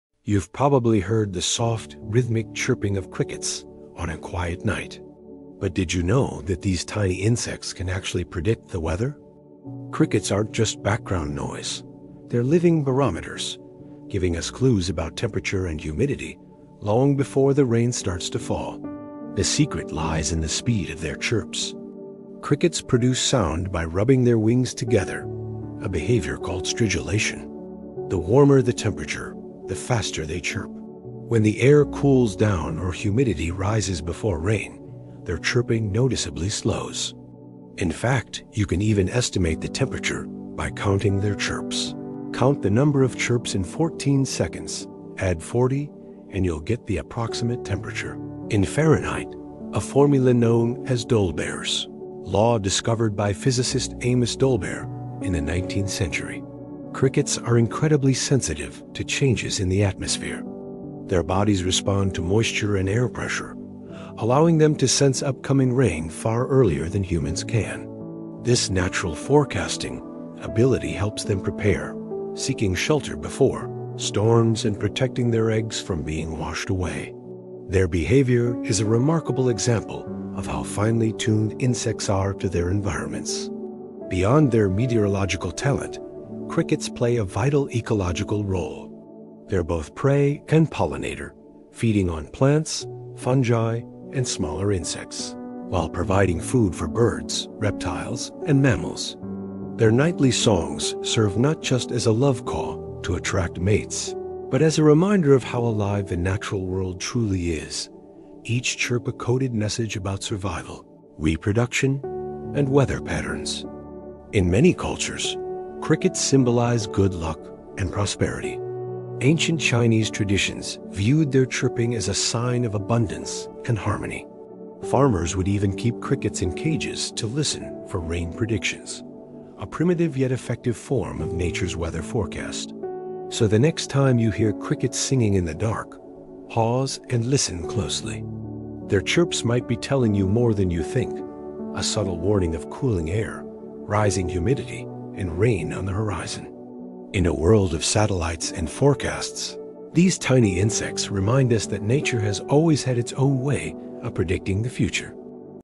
This Insect Predicts the Rain – Cricket Behavior Facts
Did you know crickets can predict the weather? Discover how their chirping speeds reveal temperature changes and upcoming rain. Nature’s smallest weather forecasters might just surprise you! CricketFacts AnimalBehavior InsectSecrets NatureWonders WildlifeFacts WeatherPrediction IncredibleInsects CricketSounds RainPrediction AnimalScience @AmazingAnimalFacts-k3y